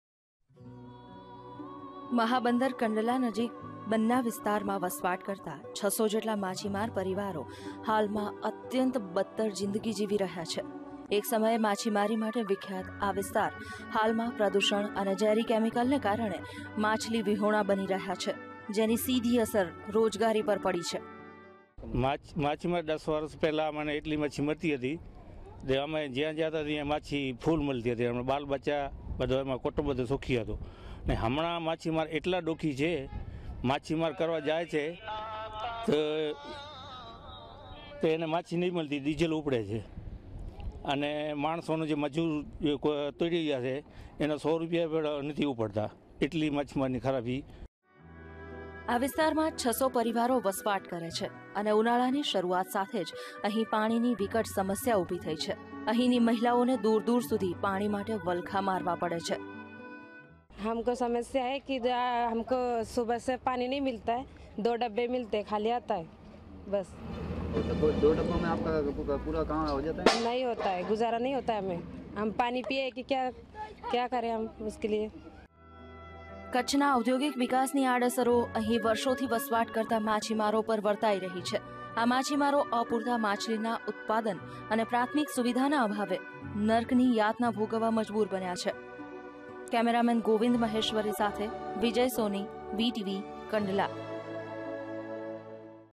VTV - FISHERMEN DEPRIVED OF PRIMARY FACILITAED AT KANDLA, KUTCH
કંડલા સ્થિત માછીમારો હાલમાં બદતર જીંદગી જીવવા મજબુર બન્યા છે. અહીના 600 જેટલા પરિવારોને પાણી અને આરોગ્ય સહિતની પ્રાથમિક સુવિધાને અભાવે પીડા ભોગવી રહ્યા છે.